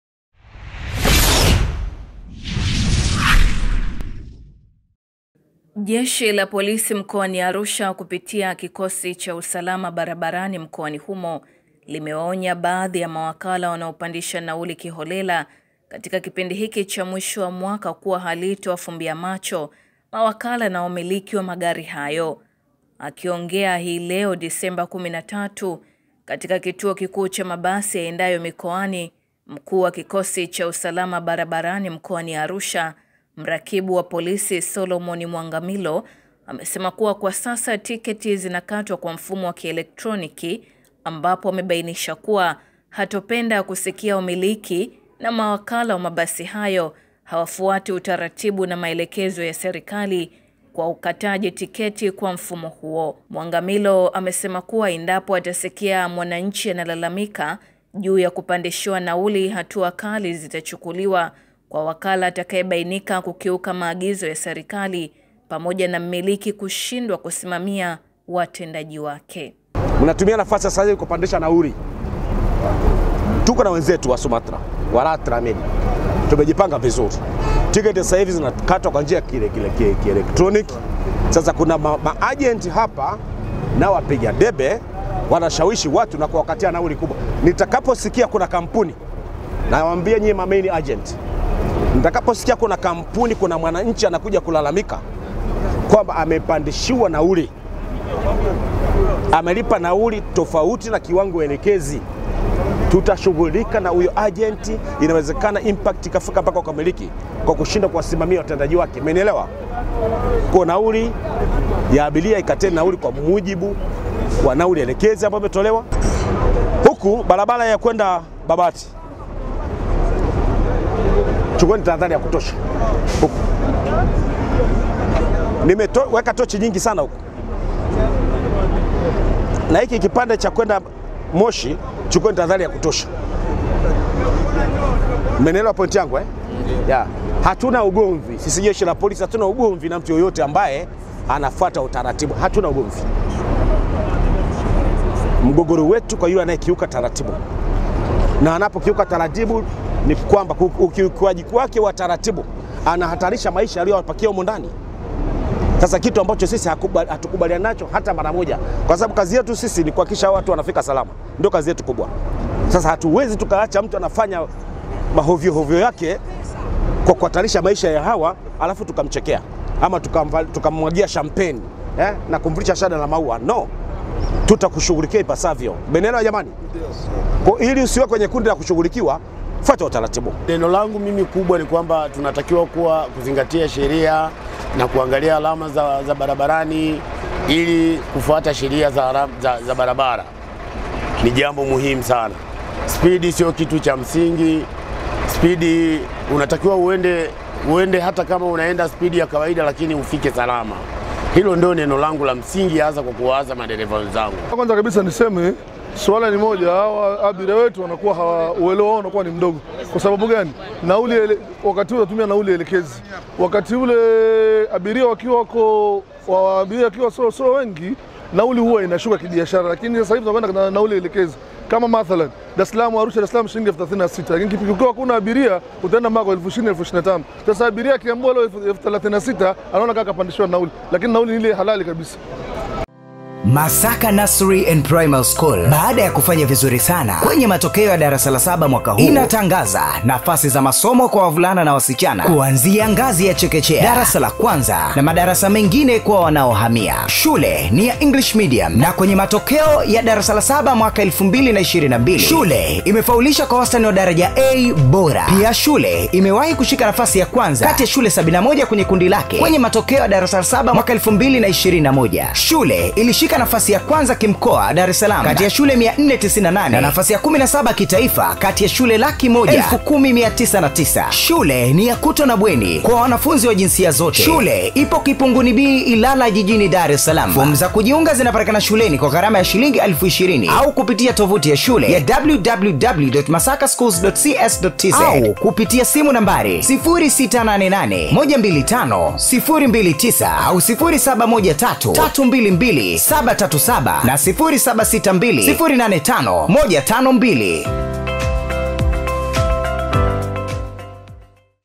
WANAOPANDISHA NAULI za MIKOANI KUKIONA cha MOTO, JESHI la POLISI LAFANYA UKAGUZI WA KUSHTUKIZA...
Jeshi la Polisi Mkoa wa Arusha kupitia kikosi cha usalama barabarani mkoani humo limewaonya baadhi ya mawakala wanaopandisha nauli kiholela kipindi hiki cha mwisho wa mwaka kuwa halitawafumbia macho mawakala hao na wamiliki wa magari hayo.

Akizungumza mapema leo Desemba 13. 2022 katika kituo kikuu cha mabasi yaendeayo mikoani, Mkuu wa Kikosi cha Usalama Barabarani Mkoa wa Arusha, Mrakibu wa Polisi, SP SOLOMON MWANGAMILO amesema kuwa, kwa sasa tiketi zinakatwa kwa mfumo wa kieletroniki ambapo amebainisha kwamba hatapenda kusikia wamiliki na wakala wa mabasi hayo hawafuati utaratibu na maelekezo ya Serikali kwa ukataji tiketi kwa mfumo wa kieletroniki.